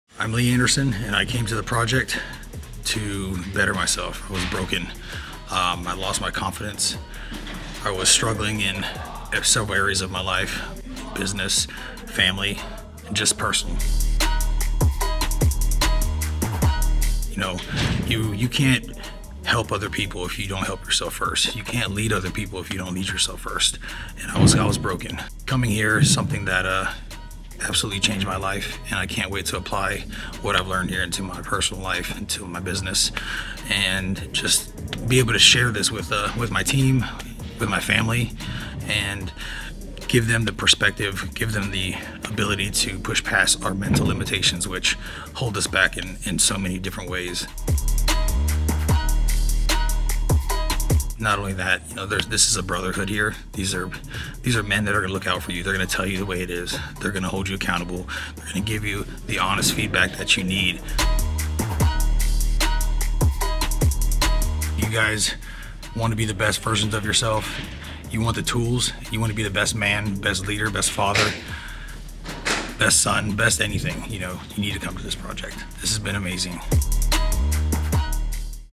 Project Testimonial- Lee Anderson
The Project- Testimonial- Lee Anderson- Men's Personal Development/ Leadership Training

The Project is a fully immersive 75 Hour experience for Men where you get the chance to Live, Train, and Learn- Physically, Mentally, and emotionally with a US Marine, a Navy SEAL, Swat operator, MMA fighter, and an Empire builder, learning to kill your inner bitch and unleash the beast, so that you can Dominate in your family, fitness, finances, and faith, becoming part of a brotherhood of men just like you, ultimately being forged into a Modern Day Knight.